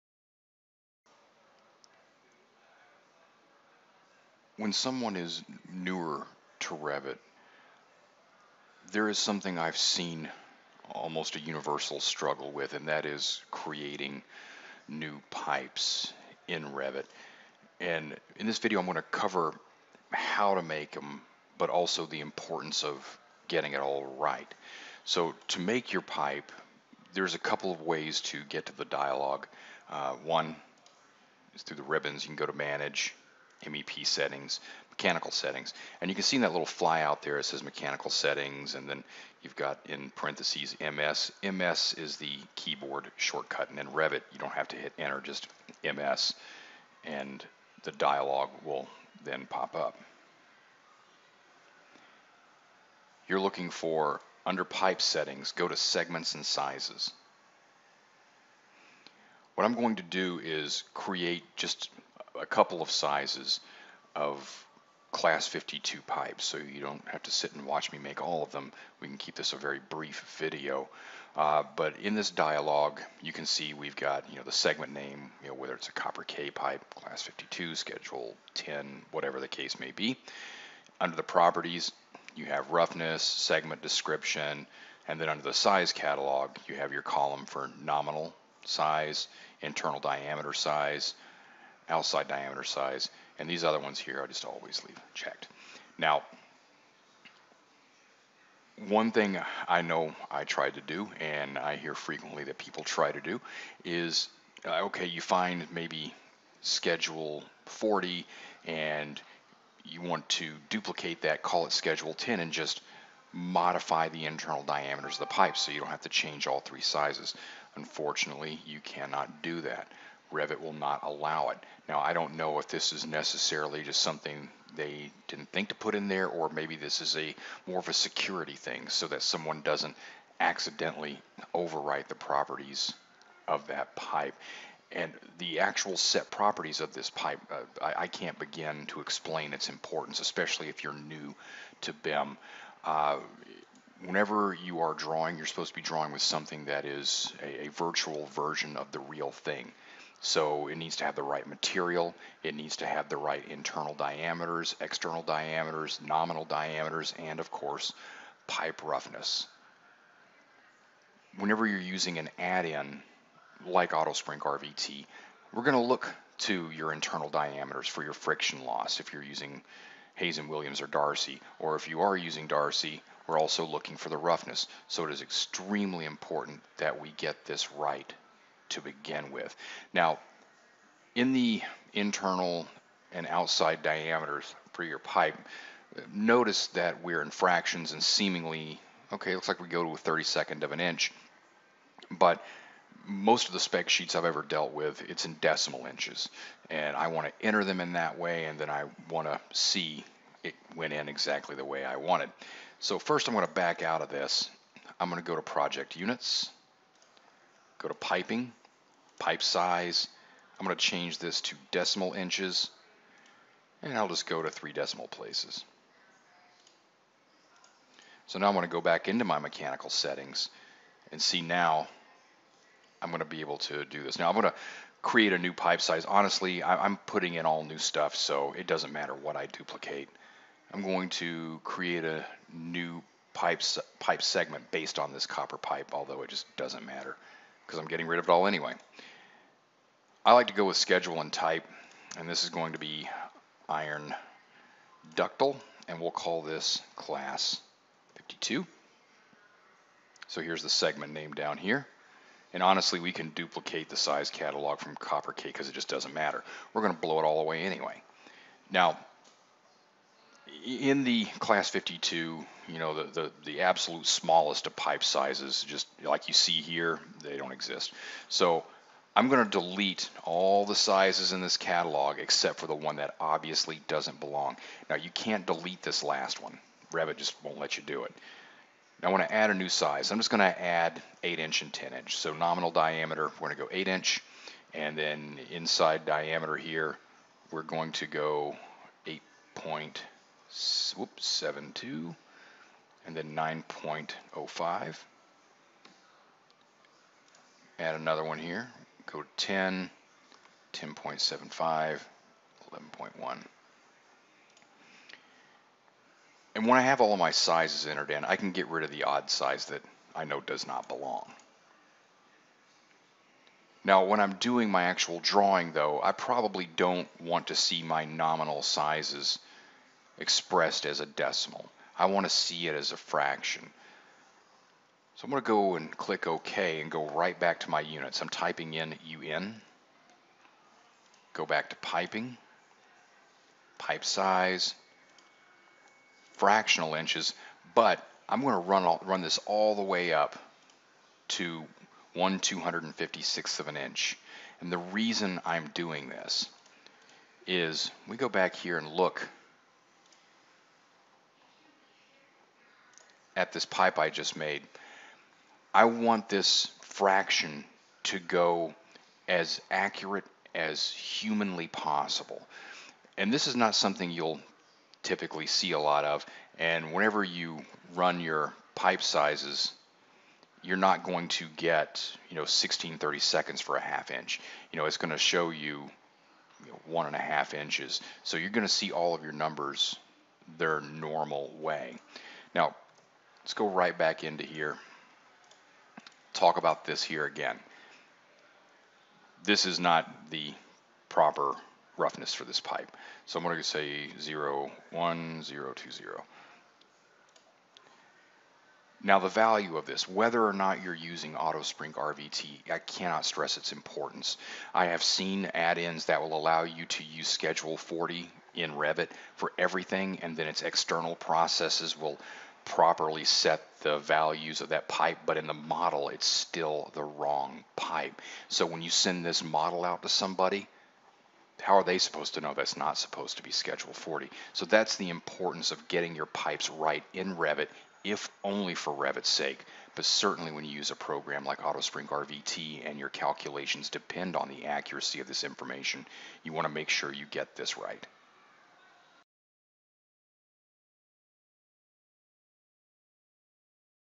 Revit: Creating New Pipe and Why You Should Care
There is a struggle I see common to most designers new to using Revit: creating new pipe. Even when they find the proper dialog to create the new pipe, they inevitably get frustrated with what it seems you can't do. Believe me, I understand. The struggles I hear people I have, I had the very same struggles. This video shows where and how to create new pipe why it is very, very important to get this information correct before you ever draw pipe in a project.